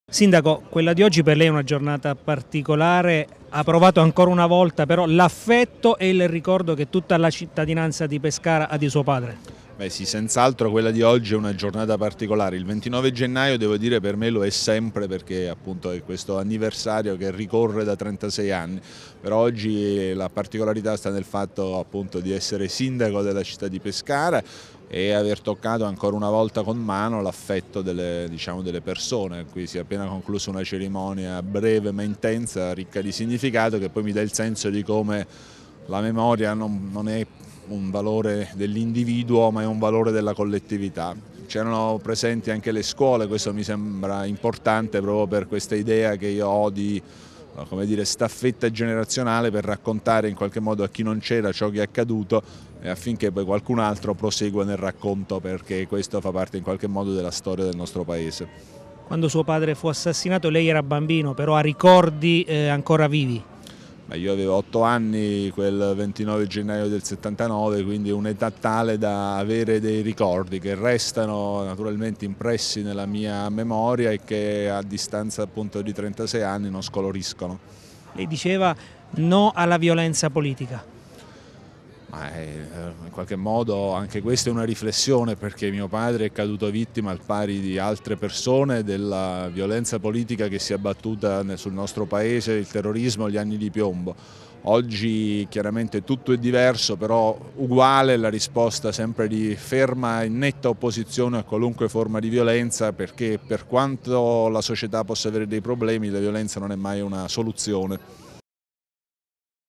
36° Anniversario dell'omicidio Alessandrini
Si é tenuta questa mattina in Piazza Unione a Pescara la deposizione di una corona in ricordo del 36esimo anniversario dell'uccisione del giudice Emilio Alessandrini